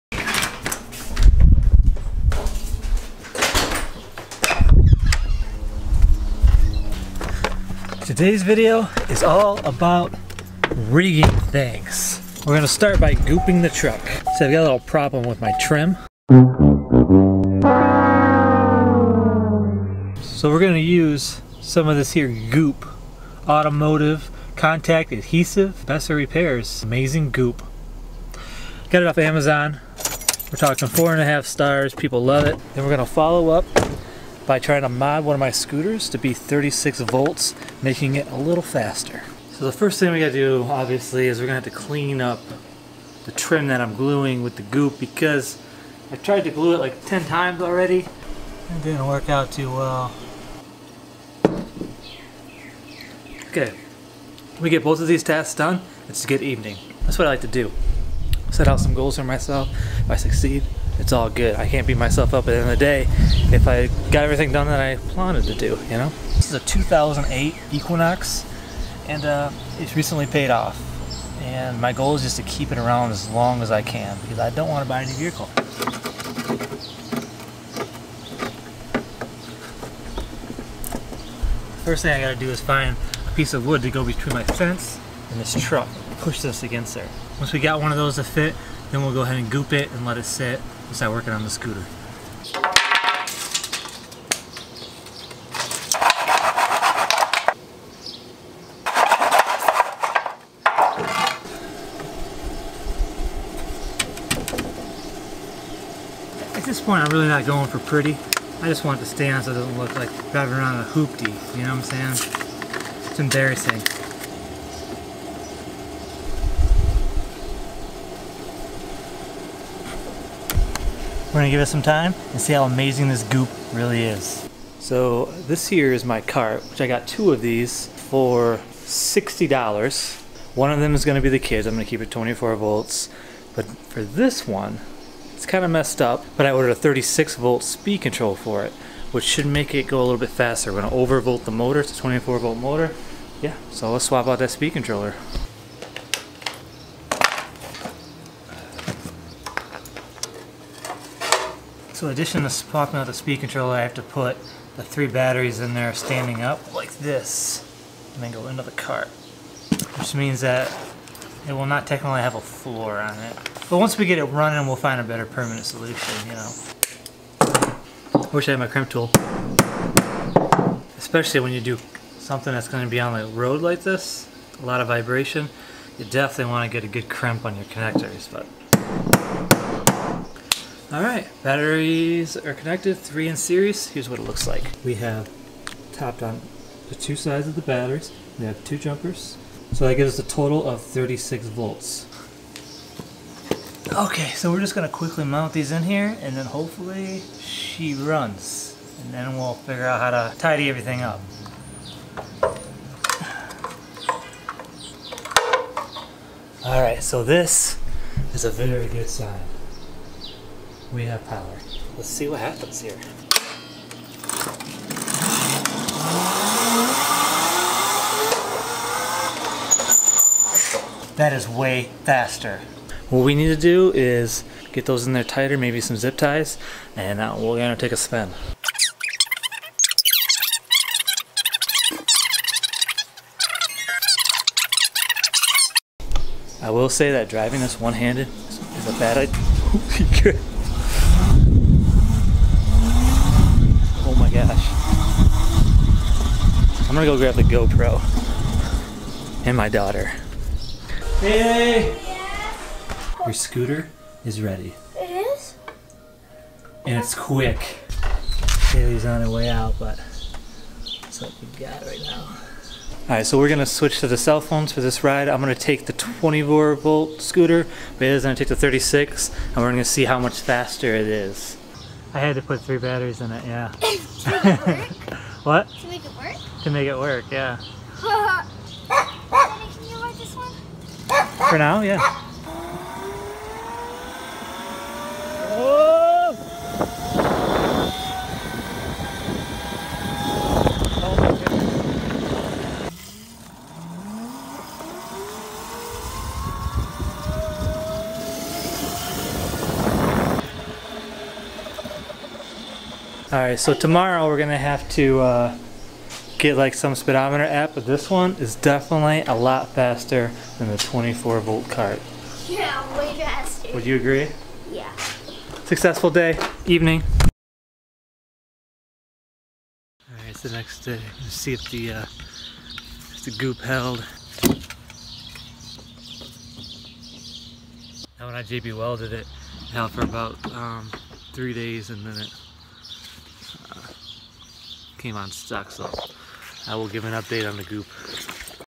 PV01 | Schwinn S500 36 VDC mod and other evening things
Practice Vlog 01. Shot in an evening edited on an hour. Working on them Vloggin skills.